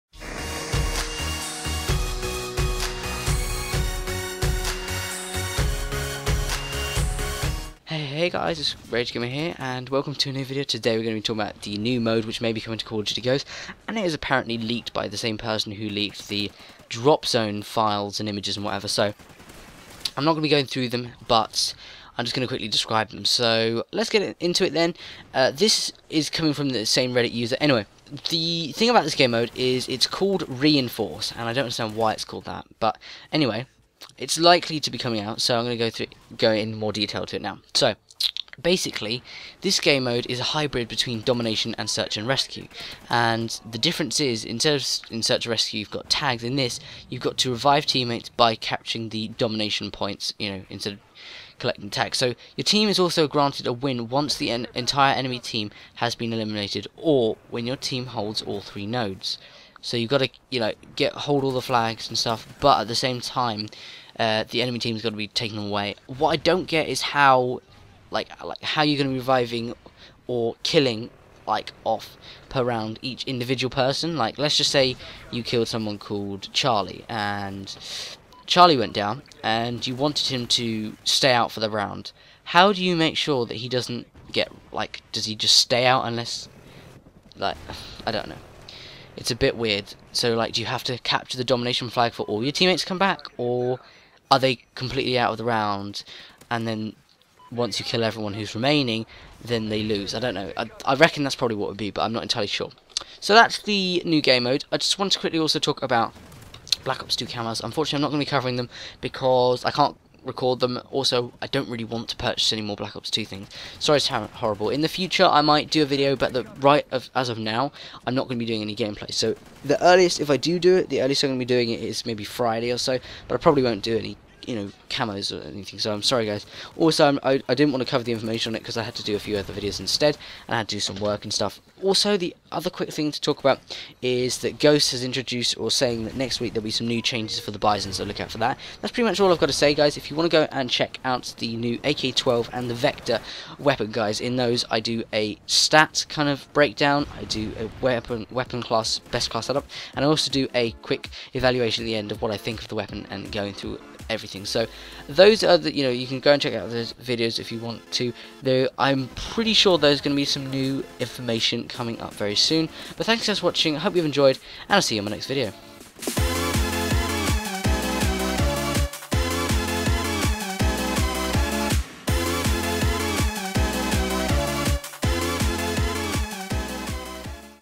Call of Duty Ghosts: NEW 'Reinforce' Game mode leaked & Coming soon?
details on the new leaked game mode that is supposedly coming to Ghosts.

Stay up to date with information!